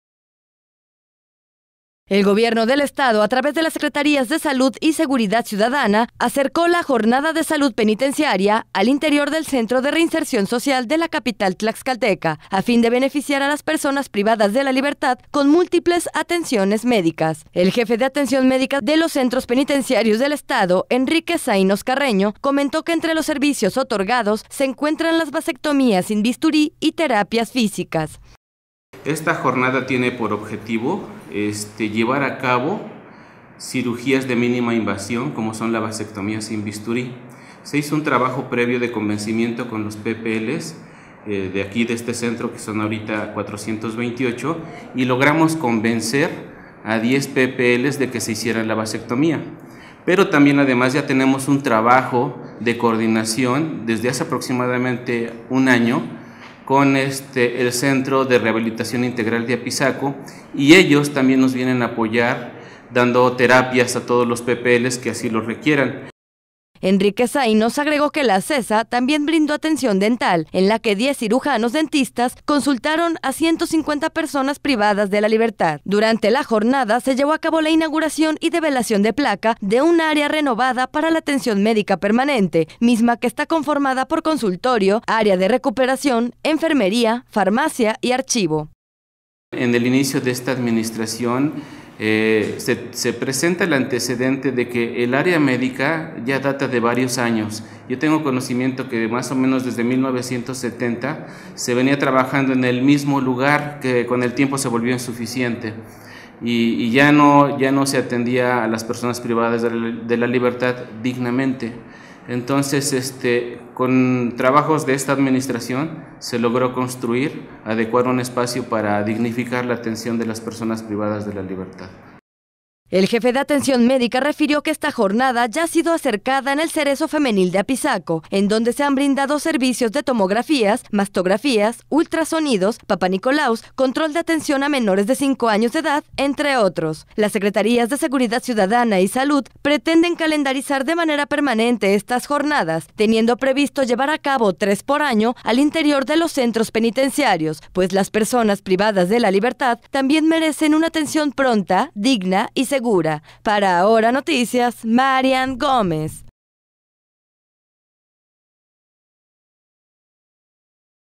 Se realiza la Jornada de Salud Penitenciaria al interior del Centro de Reinserción Social Tlaxcala
El jefe de Atención Medica de los Centros Penitenciarios del Estado, Enrique Zainos Carreño comentó que entre los servicios otorgados se encuentran las vasectomías sin bisturí y terapias físicas.